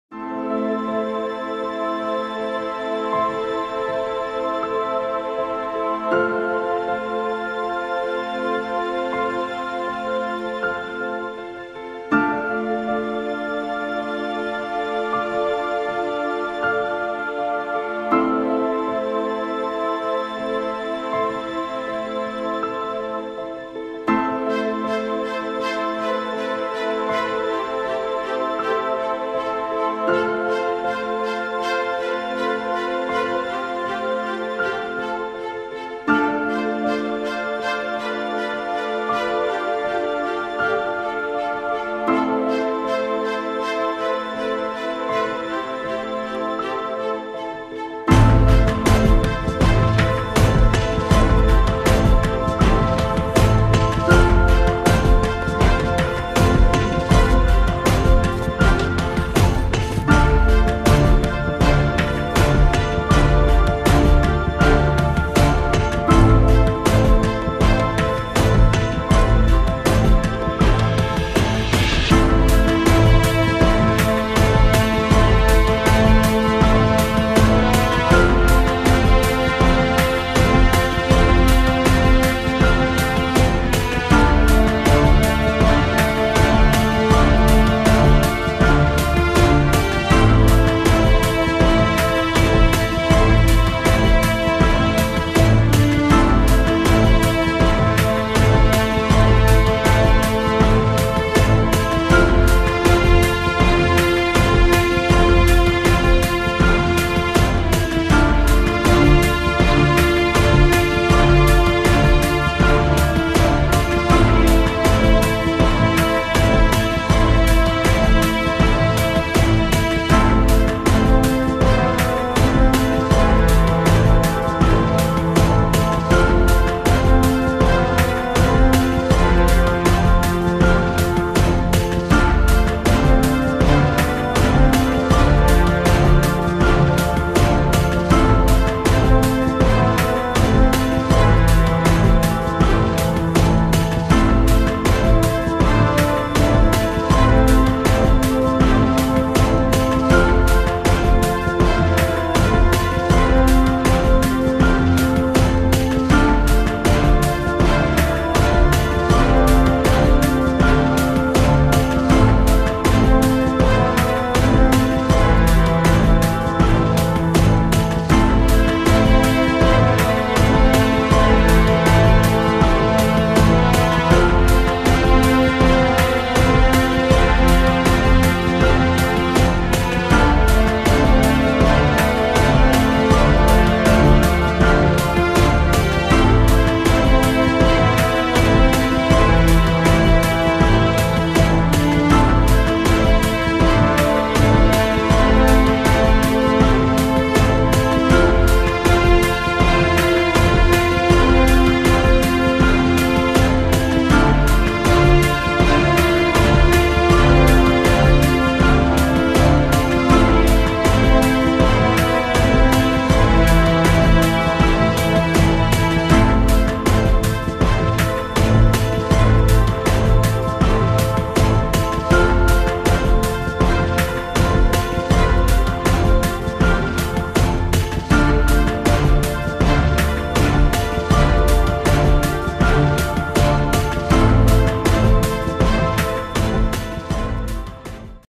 Glissement de terrain Col d'Allos / la Foux d'Allos - Automne hiver 2023
Grosses coulées de boue suite aux orages de l'automne, glissements de terrain, affaissements, dans le col d'Allos
Images du 29 décembre 2023